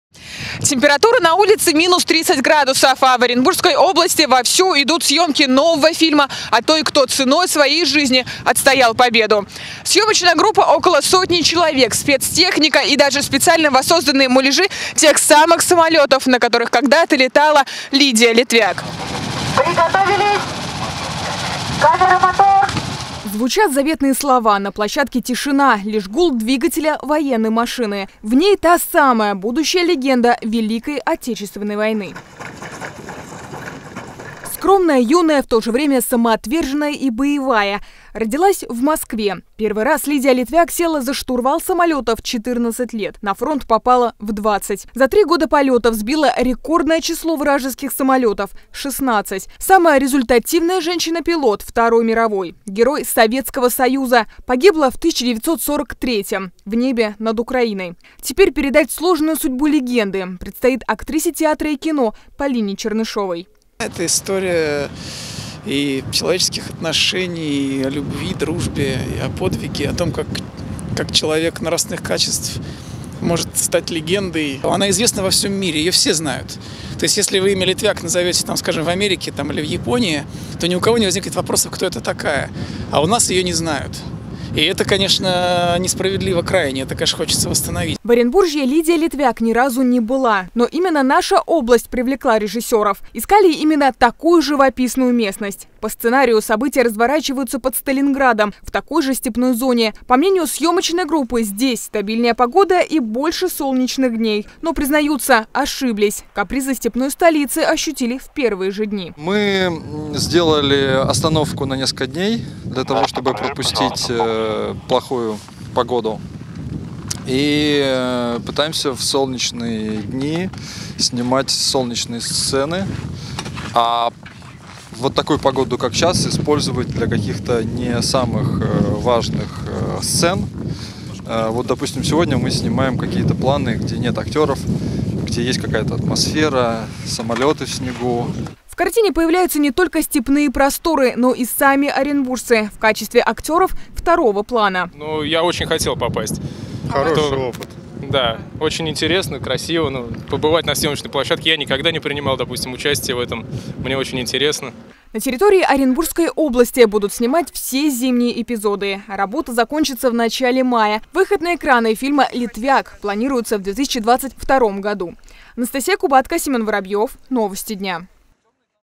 СЪЕМКИ ФИЛЬМА "ЛИТВЯК"
В Оренбургском районе начались съемки фильма о легендарной летчице. Для этого даже создали новый аэродром. Почему именно наша область привлекла режиссёров и чем знаменита Лидия Литвяк?